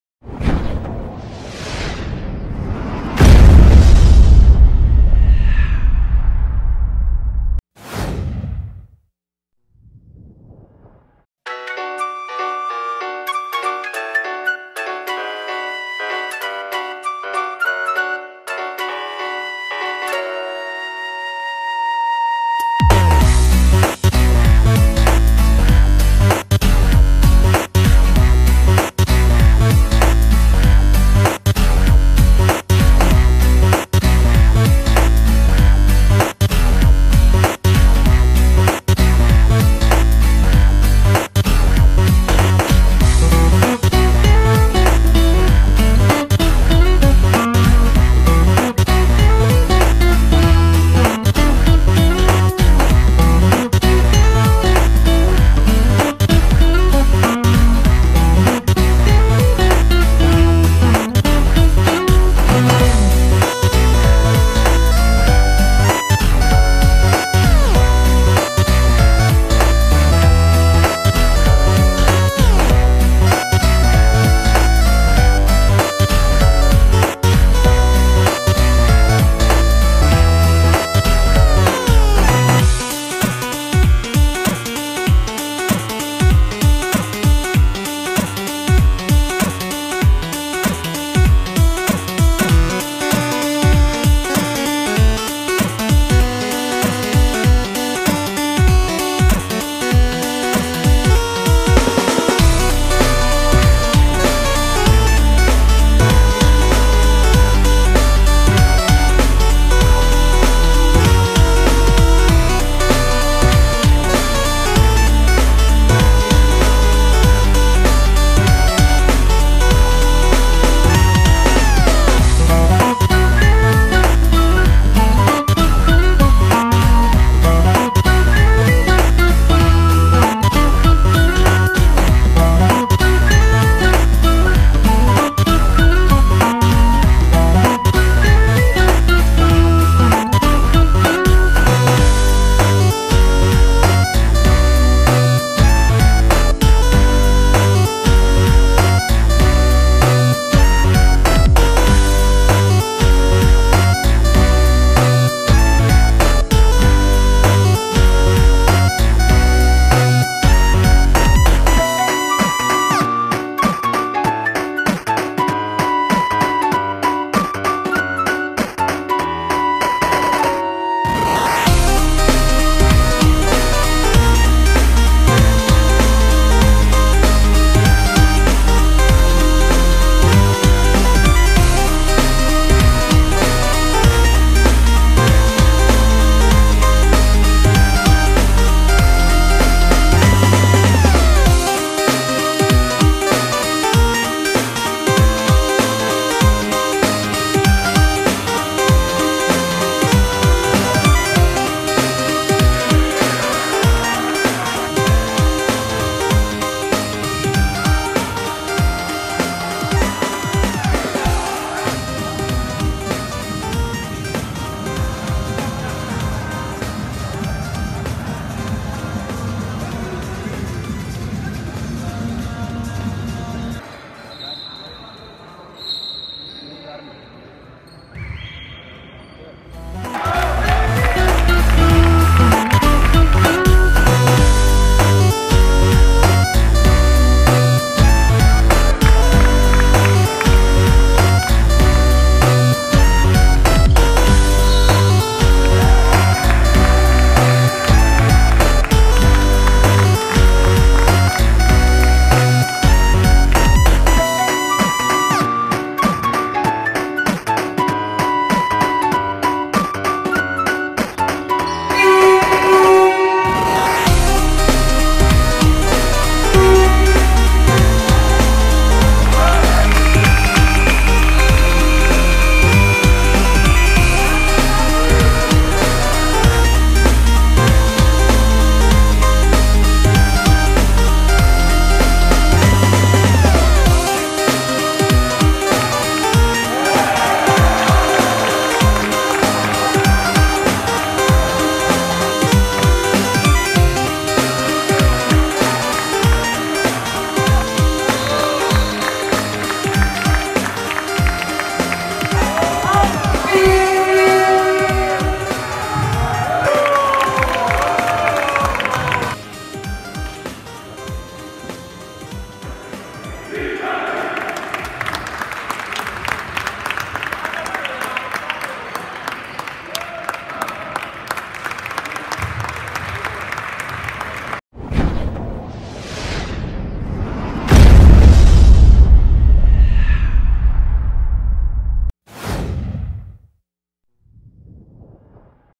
Game Film | ΧΑΝΘ vs ΙΚΑΡΟΙ 68-74
Video που καταγράφει την παρακάμερα του αγώνα ΧΑΝΘ-ΙΚΑΡΟΙ STIHL 68-74 για την 1η αγωνιστική, στον 2ο όμιλο της Β΄ Εθνικής 2018-19. Μια νέα παραγωγή του τμήματος επικοινωνίας & MΜΕ των Ικάρων, ώστε οι φίλαθλοι των «κυανοκίτρινων» να ζουν απο μέσα... του αγώνες της αγαπημένης μας ομάδας!